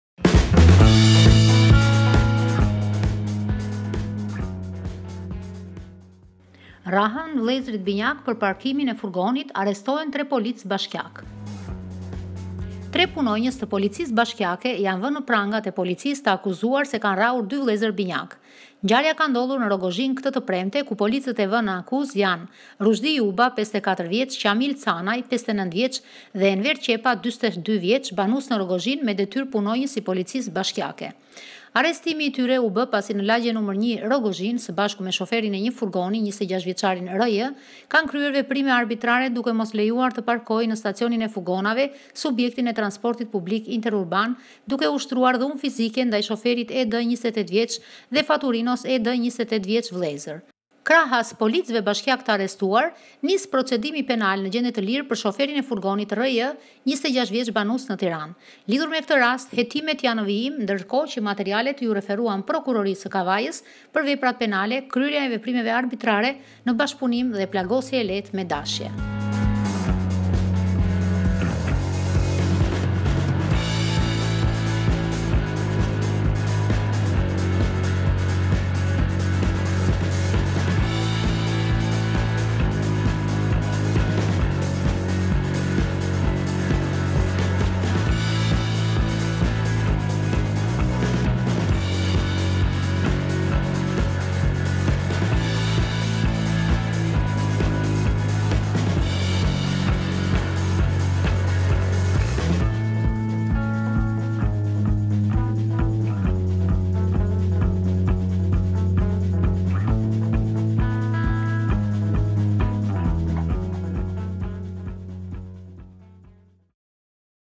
Rrahën vëllezërit binjakë për parkimin e furgonit, arrestohen tre policë bashkiakë
Tre punonjës të policisë bashkiake janë vënë në prangat e policisë, të akuzuar se kanë rrahur dy vëllezër binjak.

Ngjarja ka ndodhur në Rrogozhinë këtë të premte, ku policët e vënë nën akuzë janë Ruzhdi Juba, 54 vjeç, Qamil Canaj, 59 vjeç dhe Enver Qepa, 42 vjeç, banues në Rrogozhinë, me detyrë punonjës i policisë bashkiake.

Arrestimi i tyre u bë pasi në lagjen nr.1, Rrogozhinë, së bashku me shoferin e një fugoni, 26-vjeçarin R.J., kanë kryer veprime arbitrare, duke mos lejuar të parkojë në stacionin e fugonave, subjektin e transportit publik interurban, duke ushtruar dhunë fizike ndaj shoferit E.D., 28 vjeç dhe faturinos E.D., 28 vjeç, vëllezër.

Krahas policëve bashkiak të arrestuar, nisi procedimi penal në gjendje të lirë për shoferin e furgonit R.J., 26 vjeç, banues në Tiranë.

Lidhur me këtë rast hetimet janë në vijim, ndërkohë që materialet iu referuan Prokurorisë së Kavajës për veprat penale “Kryerja e veprimeve arbitrare në bashkëpunim” dhe “Plagosje e lehtë me dashje”. /a.y/noa.al